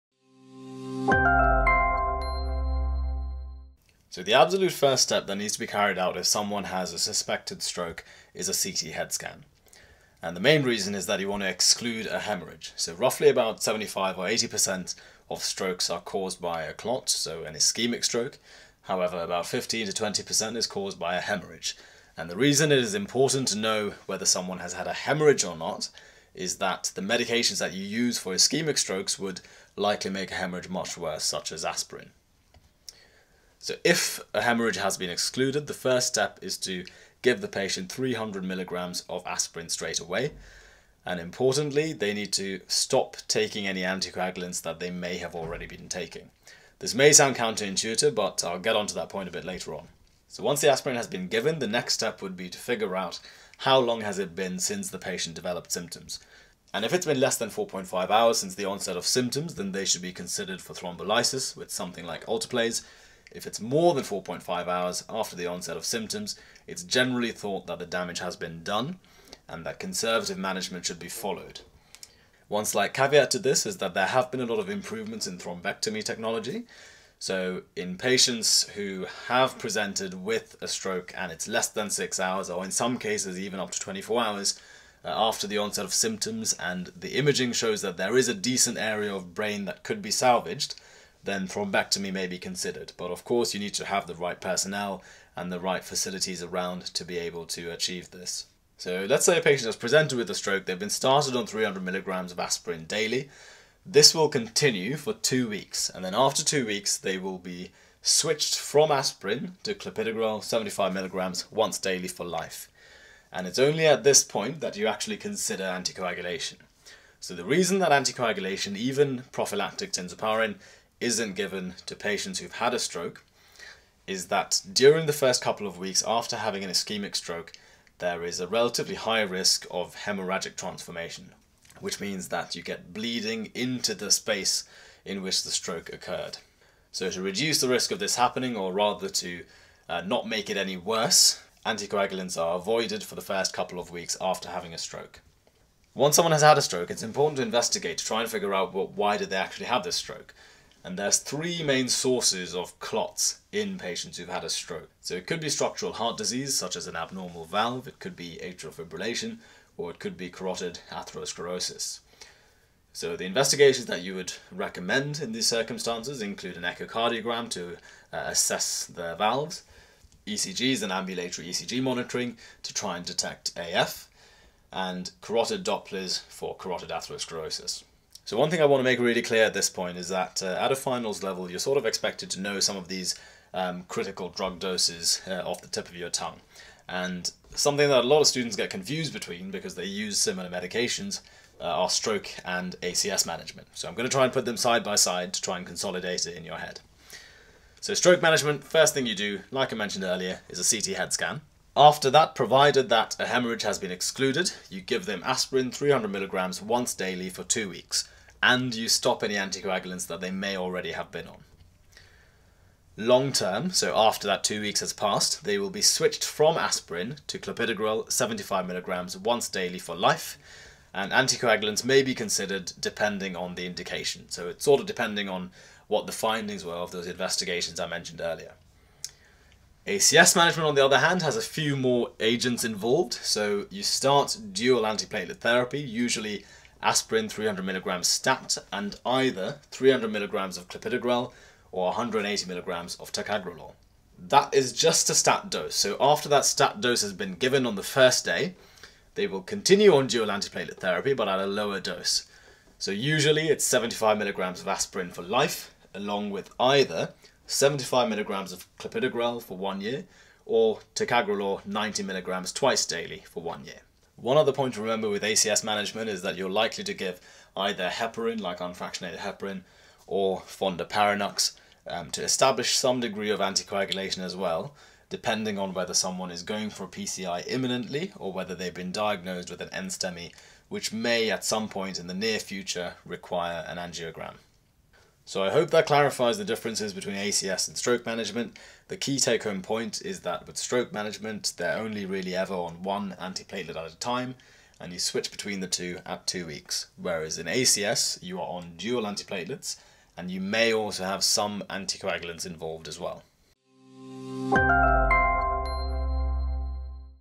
Stroke Management Explained
In this tutorial, we will be going through the acute management of stroke. We will also be comparing the management of stroke with the management of ACS, as both involve using antiplatelets and considering intervention and can often be confused.

Like this content?